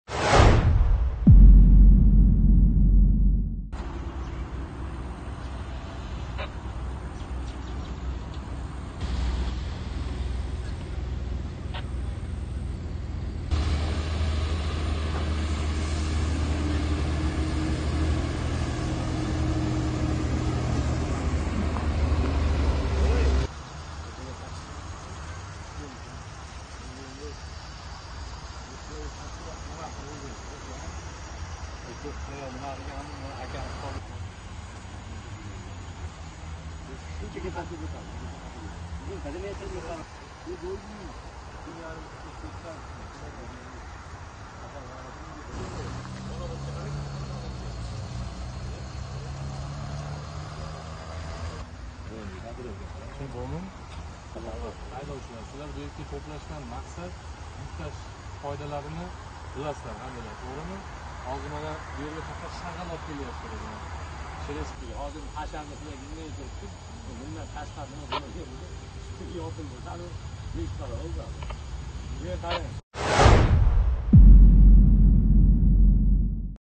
Yuk tashish bilan shug‘ullanuvchi haydovchilar o‘rtasida tushuntirish ishlari davom etmoqda
Andijon shahrida yuk tashish faoliyati bilan shug‘ullanuvchi haydovchilar o‘rtasida tushuntirish hamda reyd tadbirlari o‘tkazilishi davom etmoqda